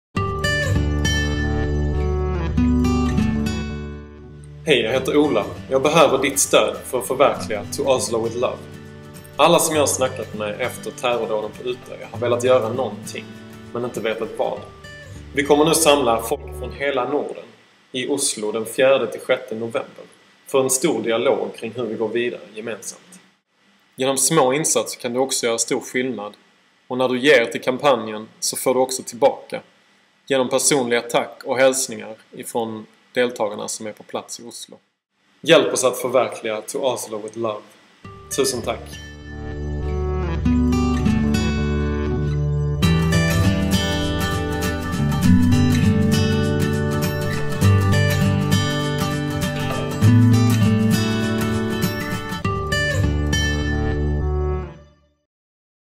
TO OSLO WITH LOVE - crowdfunding campaign video
Show your support - TO OSLO, WITH LOVE - help us gather 200 people from the Nordic region in Oslo 4-6 nov 2011, for an in depth dialog and action workshop to find the way forward after the terrorist attacks on Utoya and Oslo this summer.